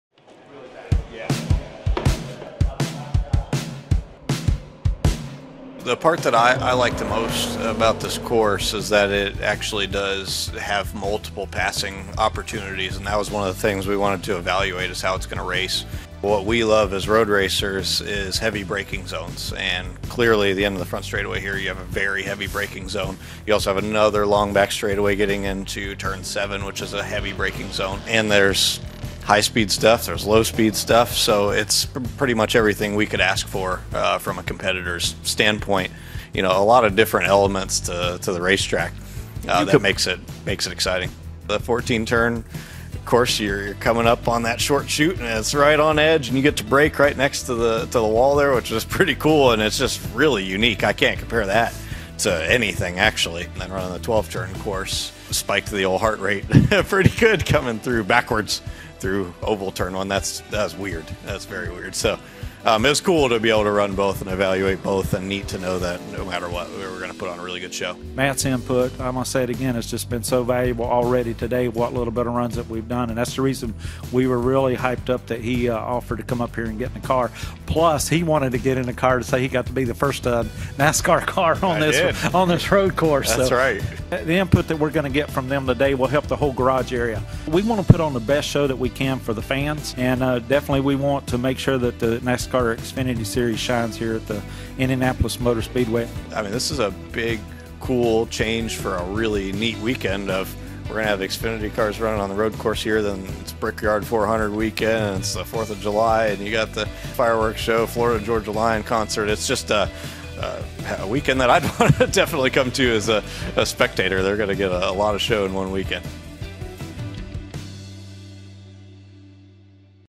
Matt DiBenedetto breaks down Indy road-course test
Hear from Matt DiBenedetto after he tested the road course at Indianapolis Motor Speedway that will be added to the 2020 NASCAR Xfinity Series schedule.

Play every weekend!